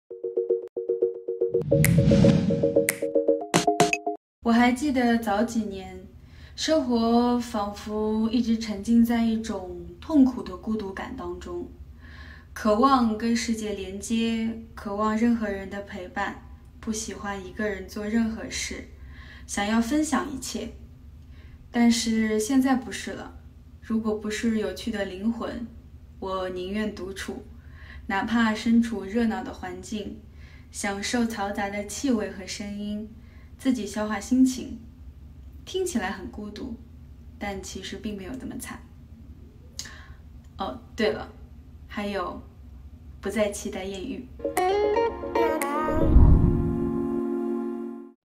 Naomi Wang (王菊) - The SECRET behind Studying Abroad
What changed her mood and the decision,
we sure believe that everyone is curious !
- Follow our channel to lock more life records

NSM (NewStyle Media) Entertainment official ：
[ Youtube ] @NewStyle Media新湃传媒 |